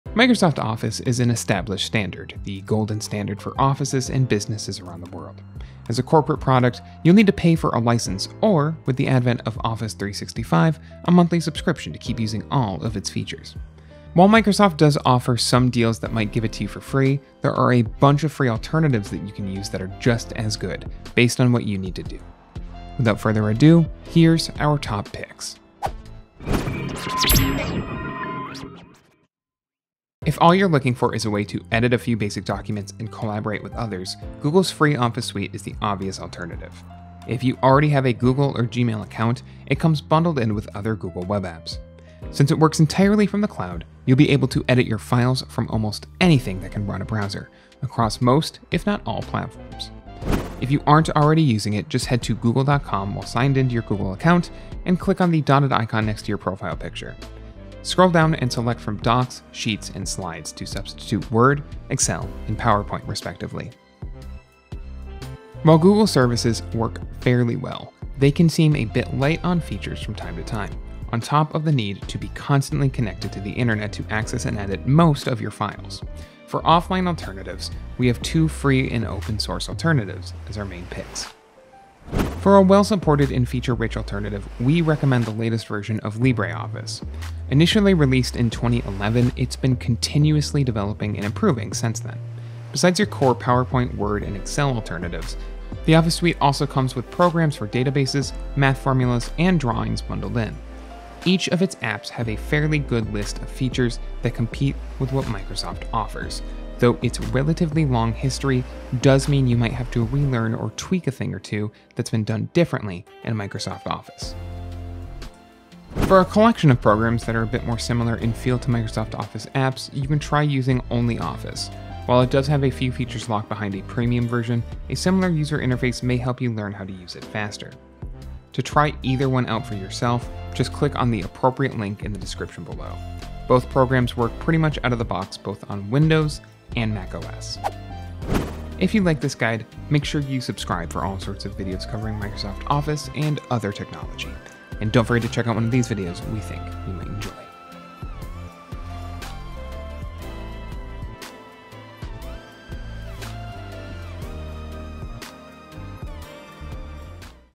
Best Free Alternatives to Microsoft Office
Microsoft Office is an established standard, the golden standard for offices and businesses around the world. As a corporate product, you’ll need to pay for a license, or with the advent of Office 365, a monthly subscription to keep using all it’s features.While you can find a Microsoft offer that gives it to you for free, there are a bunch of free alternatives you can use that can be just as good, based on what you need to do. Without further ado, here are our top picks!